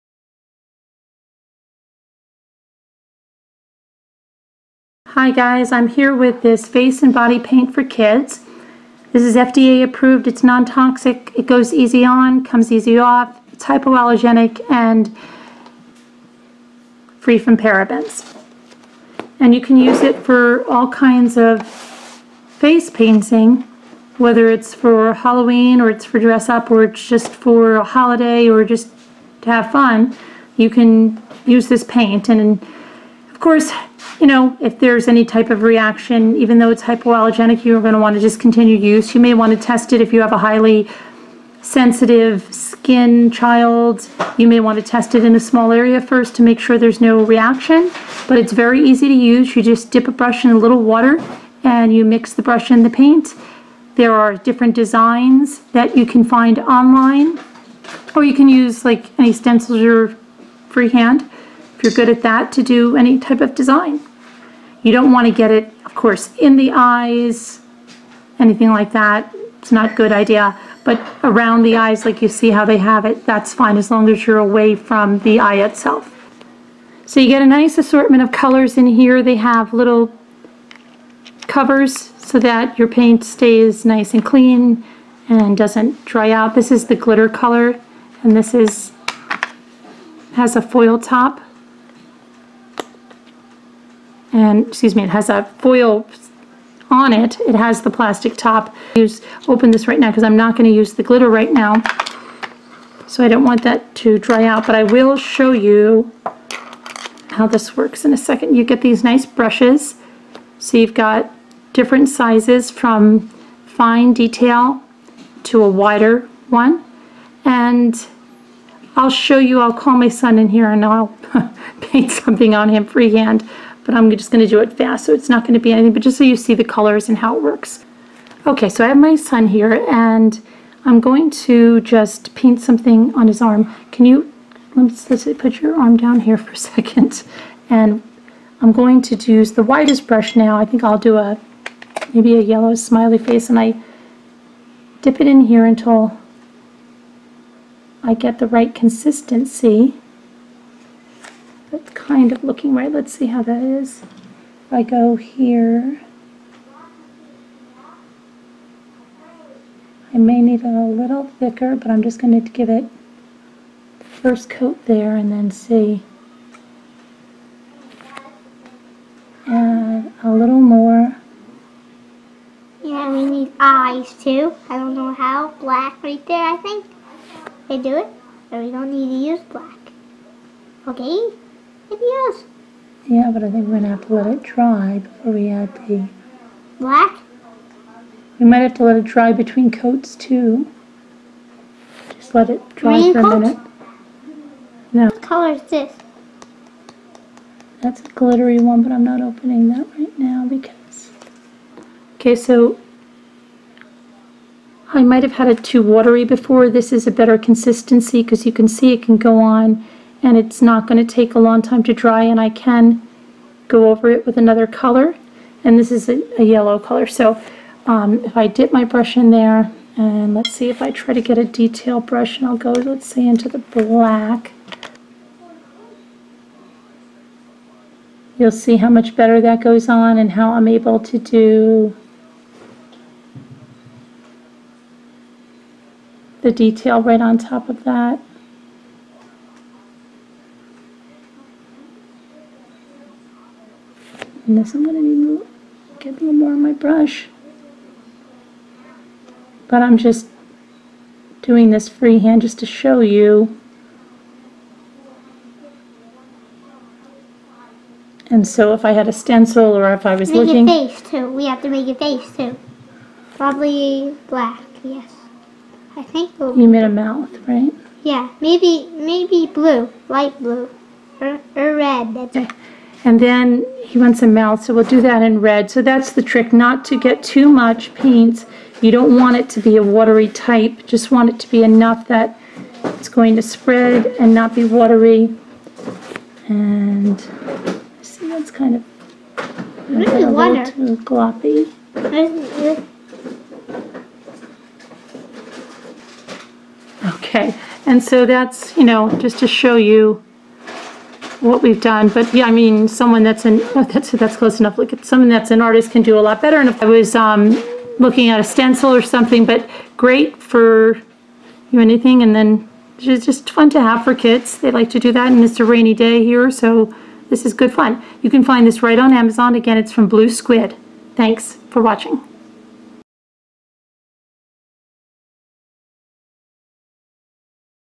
Blue Squid Face & Body Paint
MommyRamblingsBlog Mommy Ramblings Reviews The Blue Squid's Face Painting Kit is extra large with 12 colors suitable for so many different designs. This children face paint set is great for children's birthday parties, Halloween, dress up and so much more!